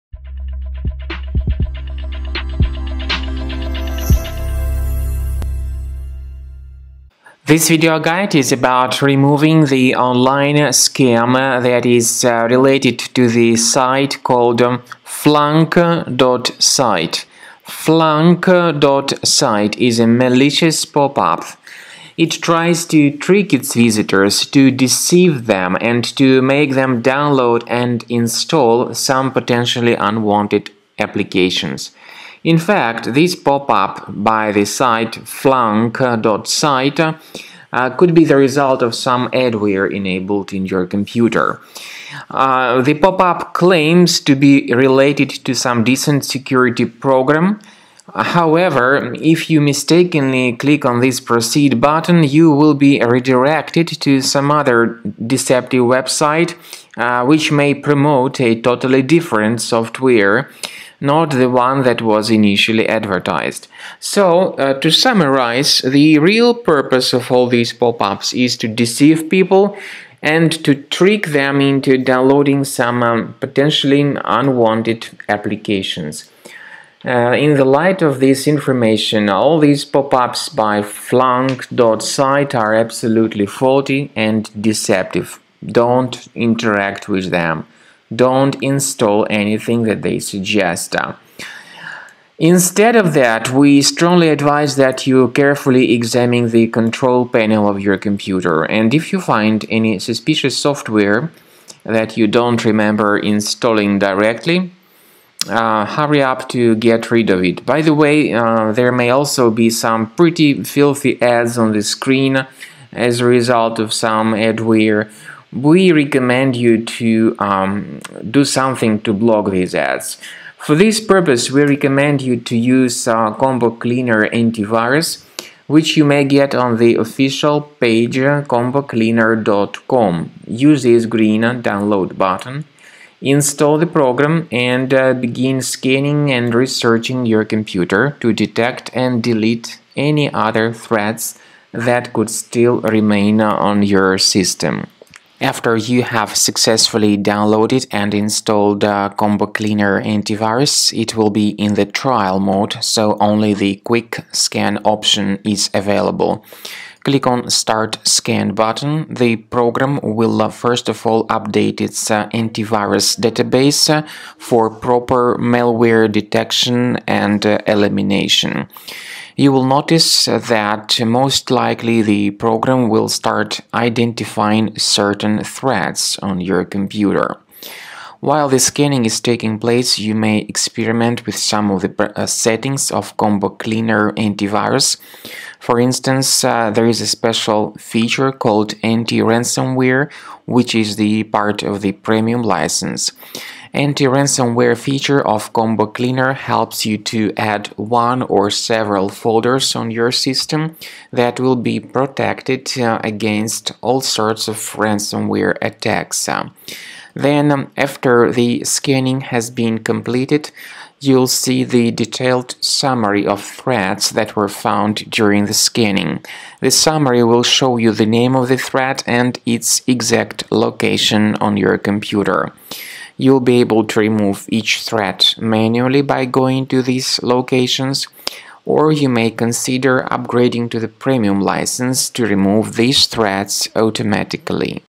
Flunk.site online scam - how to eliminate it from browser?
Flunk.site is truly a malicious site that displays the great portion of scary alerts that try to force people into downloading and installing some potentially unwanted applications. Such pop-ups, by the way, may be the trace of some adware enabled in your computer. We recommend you to be very careful not to interact with these fake alerts! Get rid of them with the help of Combo Cleaner Antivirus or follow the manual guidelines provided in the video tutorial.